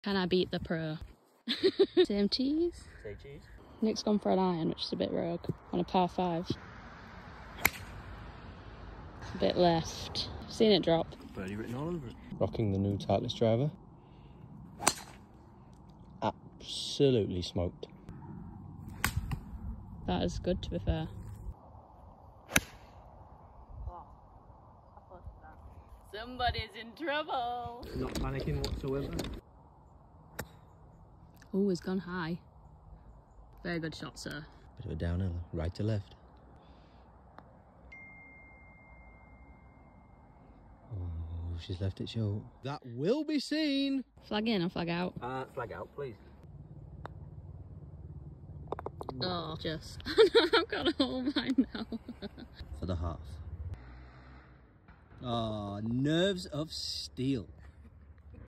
Can I Beat The Pro?
I take on Professional golfer Nick Marsh in a one-hole match on the 3rd at Huddersfield Golf Club. So can I beat the pro?